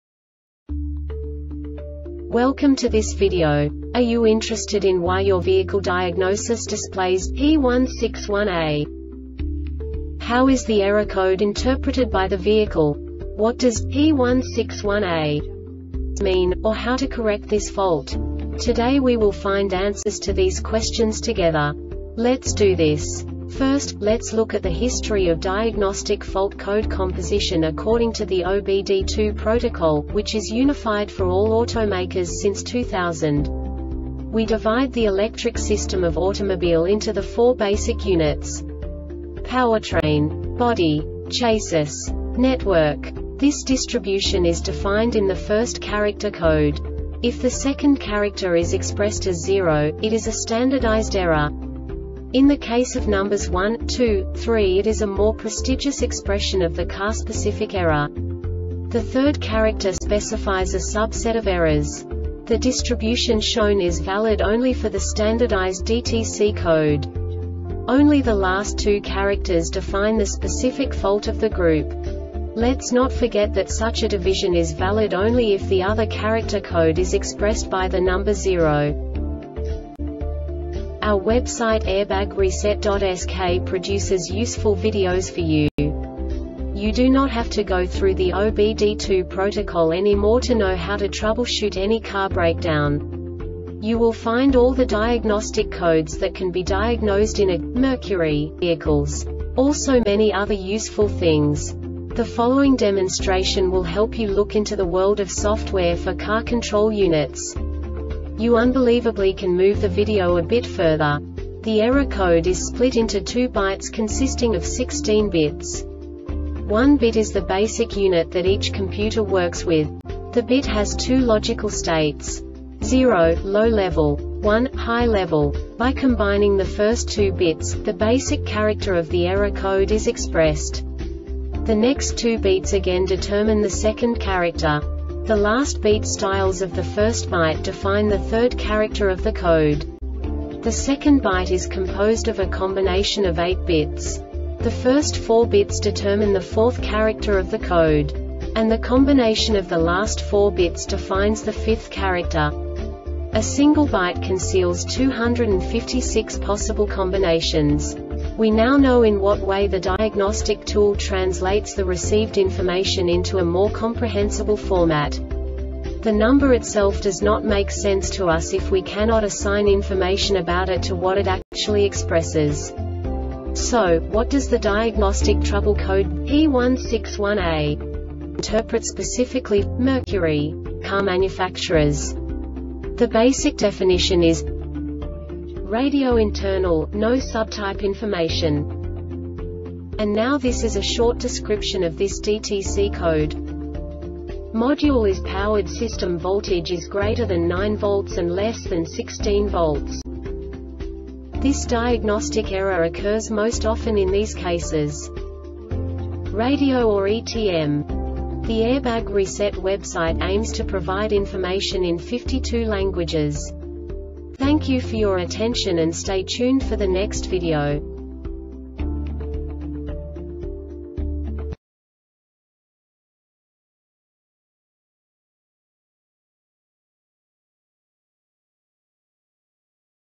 DTC Mercury P161A Short Explanation
Contents:
0:21 Basic DTC analysis according to OBD2 protocol standard.
1:48 Insight into programming
2:58 A diagnostically understandable description of the Diagnostic Trouble Code
3:05 Basic error definition

Make
Mercury
Code
P161A
Definition
Radio Internal - No Subtype Information
Description
Module is poweredSystem voltage is greater than 9 volts and less than 16 volts
Cause
Radio or Etm